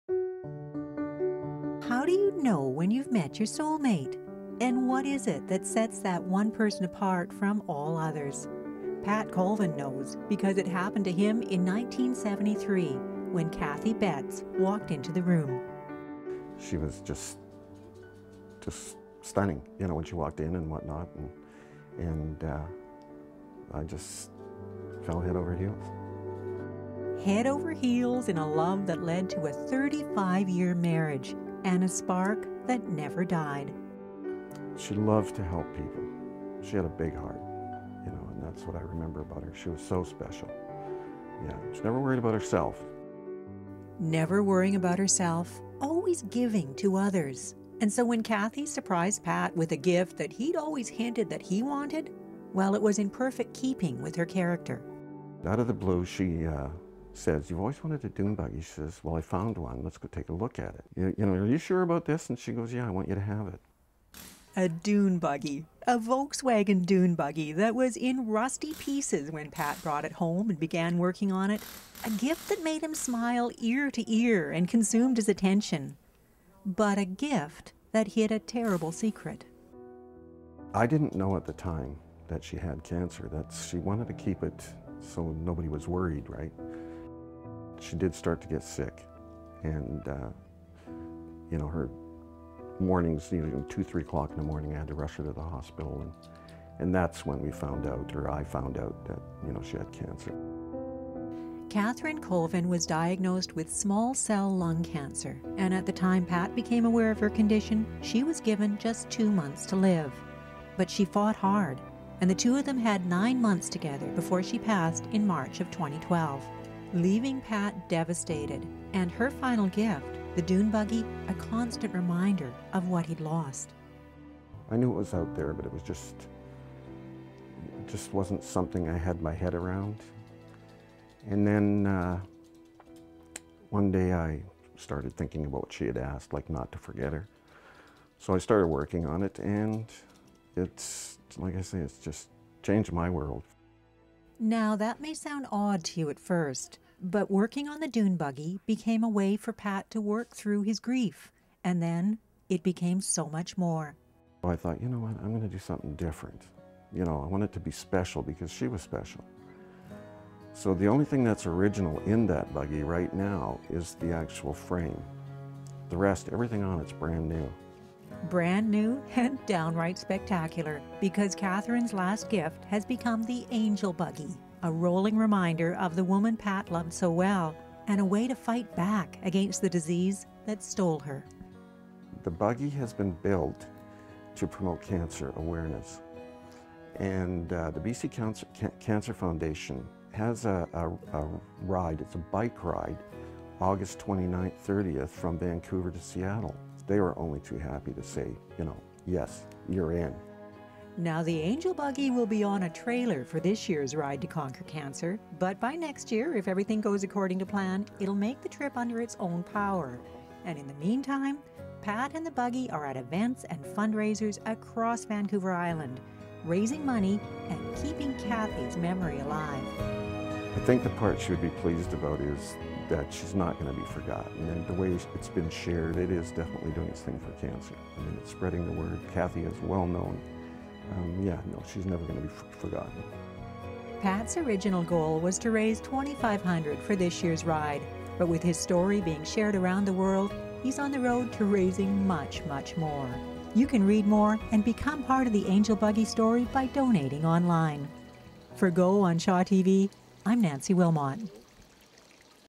Angel Buggy - Shaw TV Port Alberni
Pat Colvin loves his dune buggy...but not for the reason you might think!